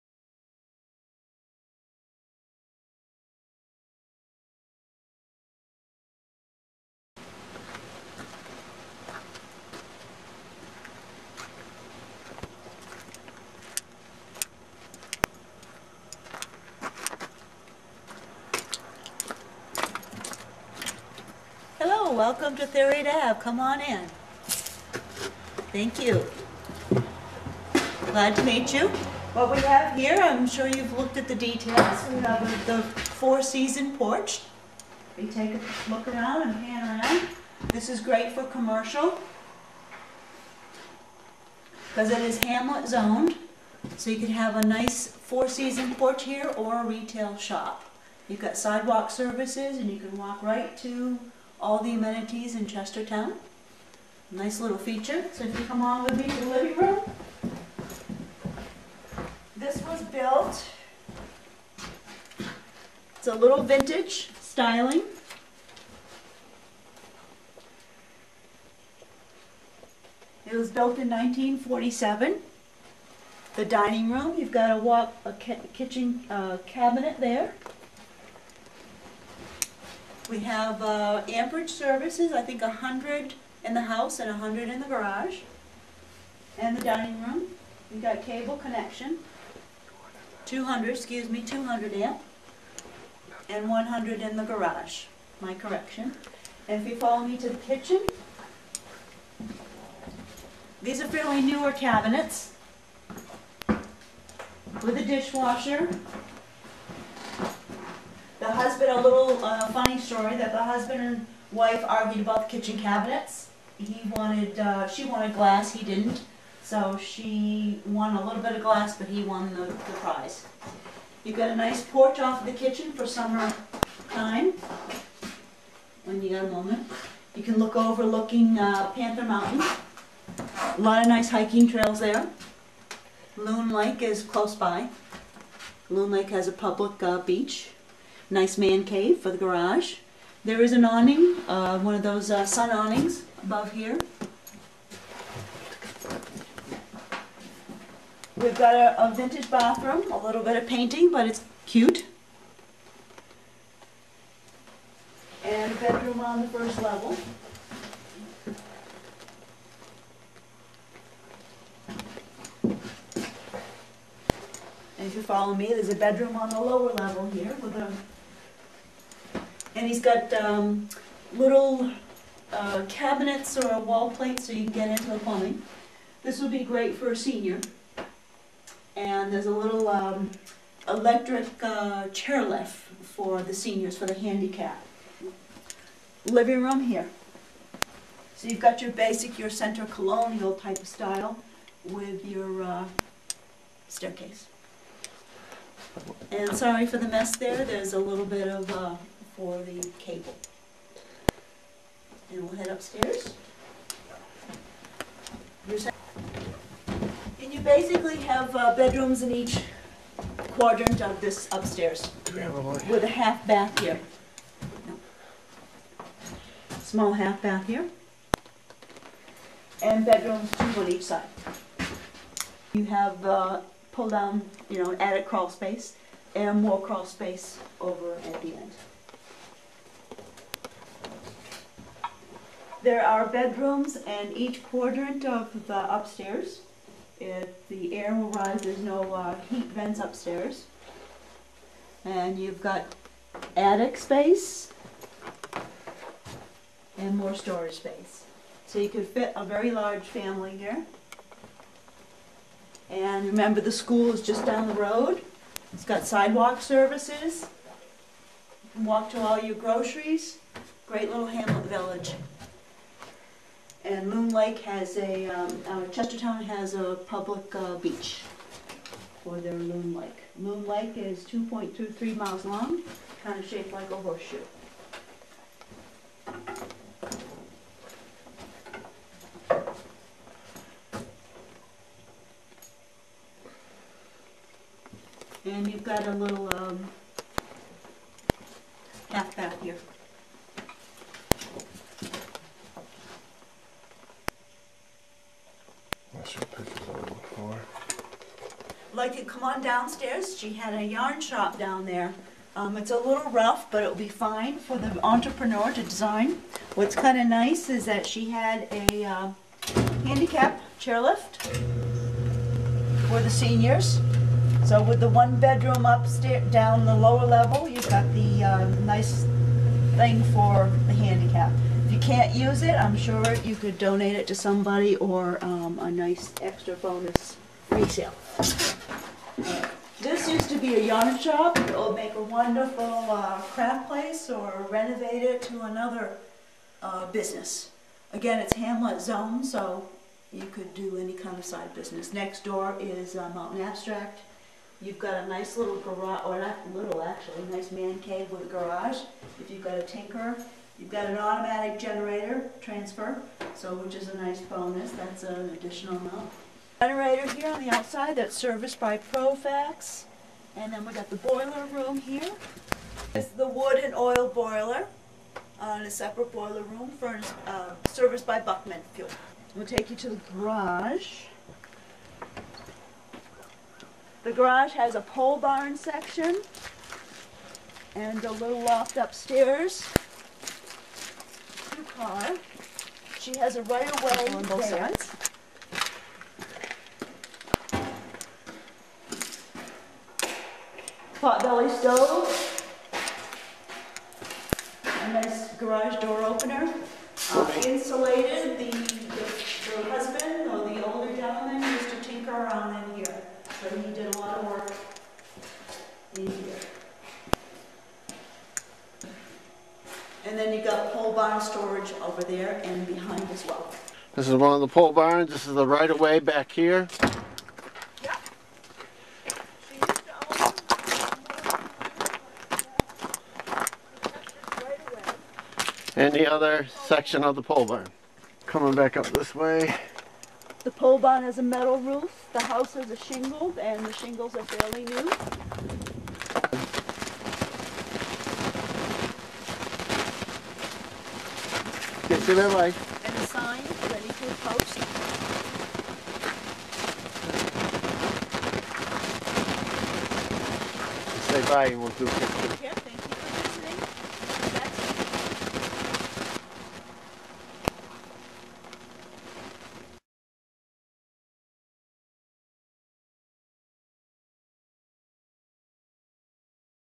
RE0488 - Video Tour with Broker
Vintage Village home in the Adirondacks.  This home is being offered by Adirondack Country Homes Realty, and is located in the town of Chestertown.  Five bedrooms 2 baths with handicapped lift, and sidewalk services.  Great for large family or seniors.  Mix usage (commercial usage).